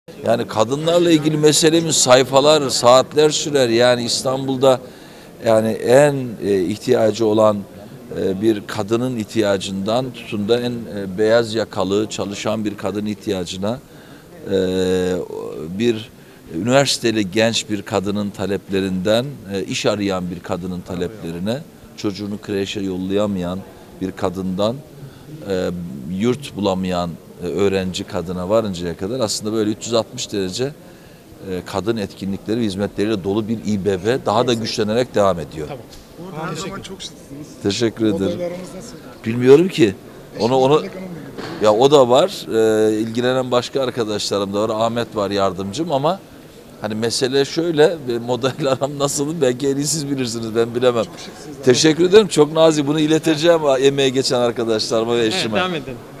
Ekrem İmamoğlu KadıncaTv Röportajı
EKREM İMAMOĞLU (İstanbul Büyükşehir Belediye Başkanı)

İstanbul Büyükşehir Belediye Başkanı Ekrem İmamoğlu , kadınlar için yapılan çalışmaları Kadınca Tv mikrofonlarına anlattı.